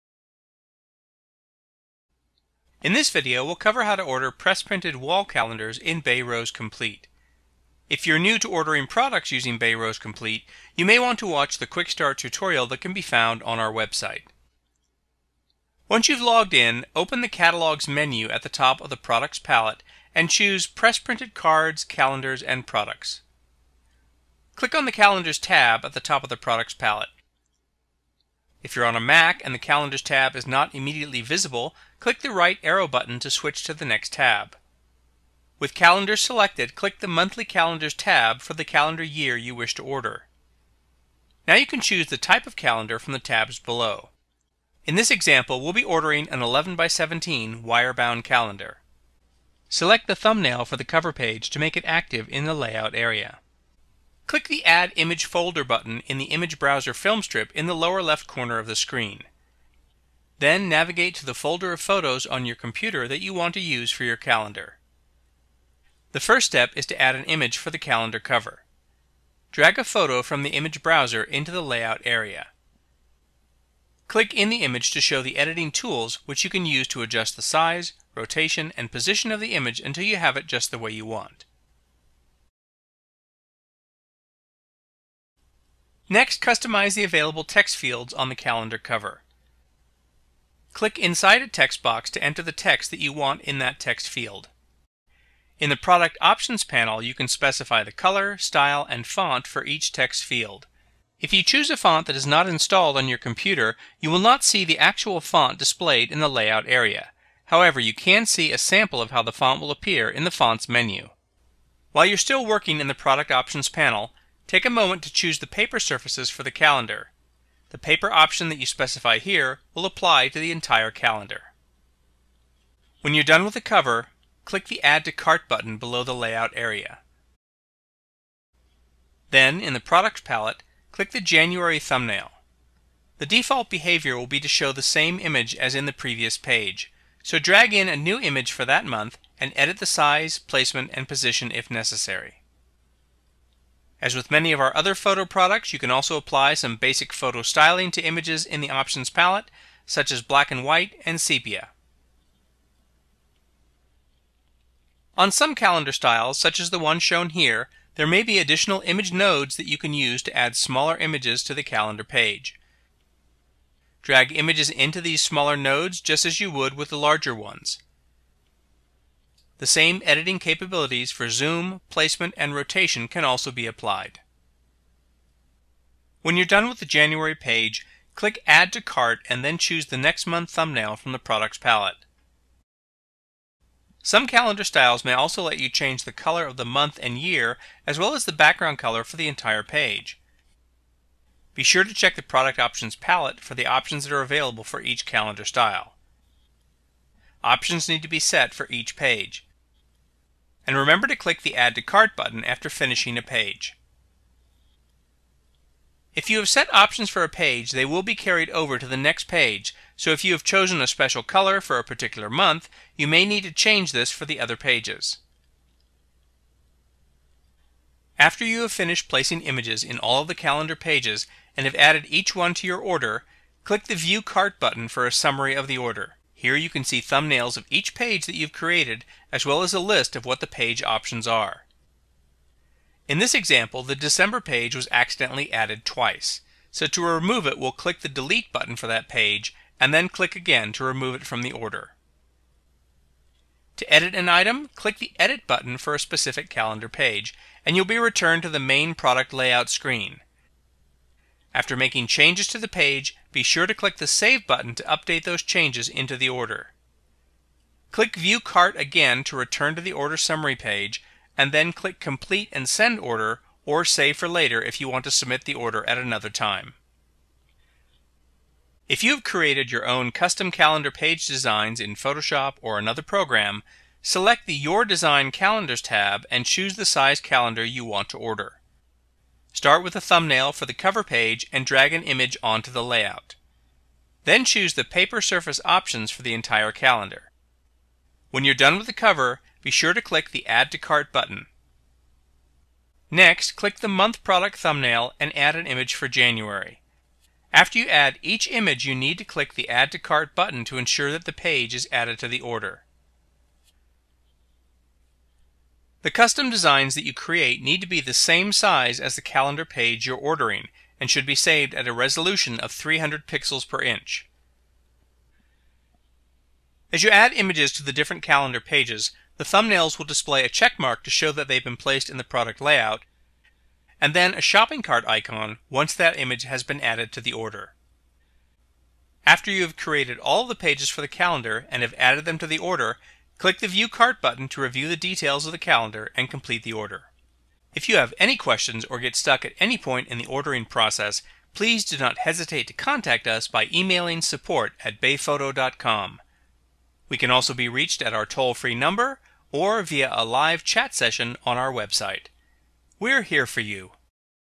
How to Order Calendars in Bay ROES
Learn how to order publisher-quality press printed calendars from your images and/or artwork using Bay ROES online ordering software.